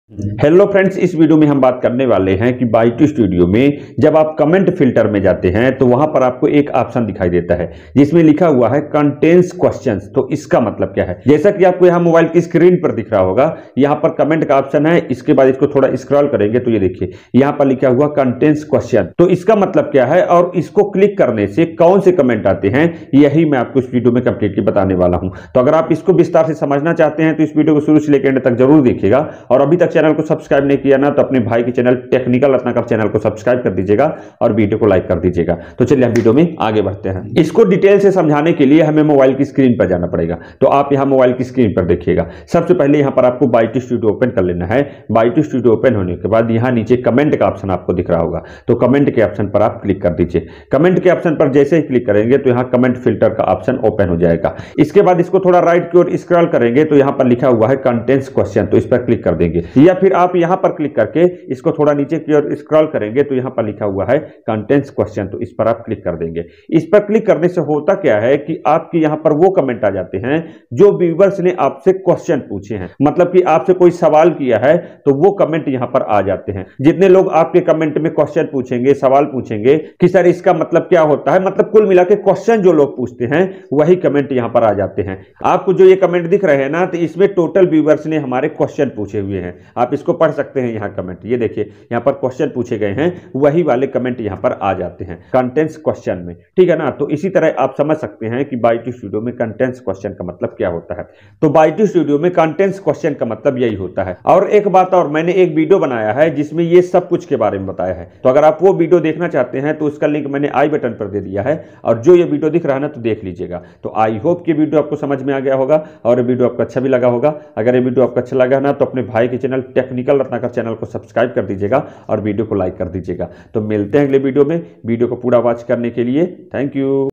YT Studio Contains Questions || Comments Filters me Contains Questions Ka Matlab Kya Hota Hai
Hello friends is video me maine aap ko yt studio me comments filters ke contains questions ke bare mein bataya hai